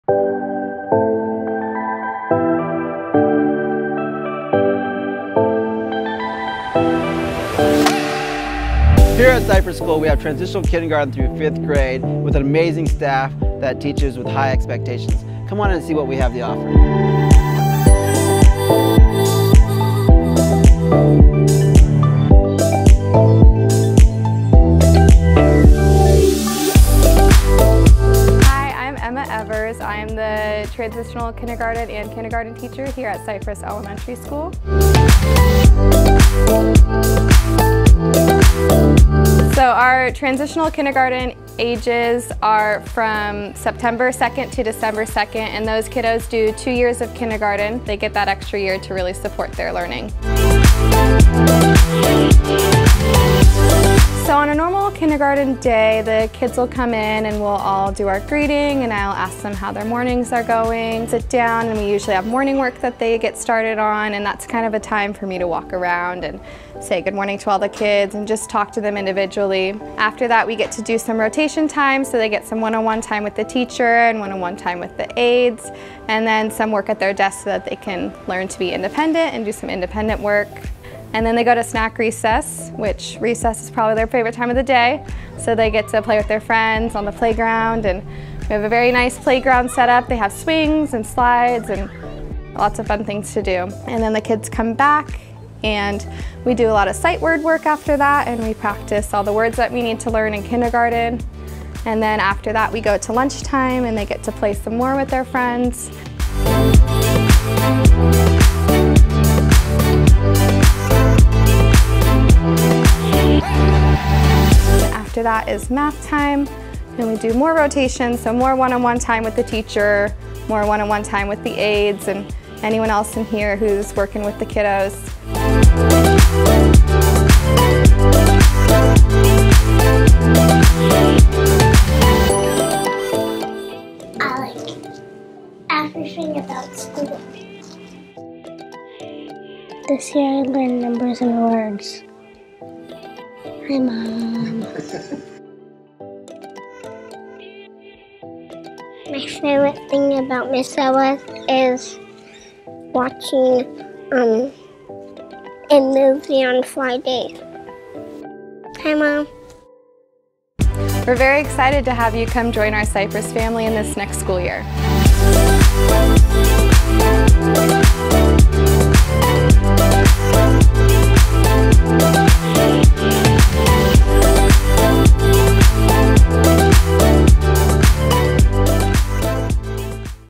Cypress Kindergarten Roundup
Cypress Elementary is a special place to start your child's TK or Kindergarten journey.  From it's small convenient location in the heart of Redding, expansive playground, scratch cooking in the kitchen and loving staff your students will feel right at home.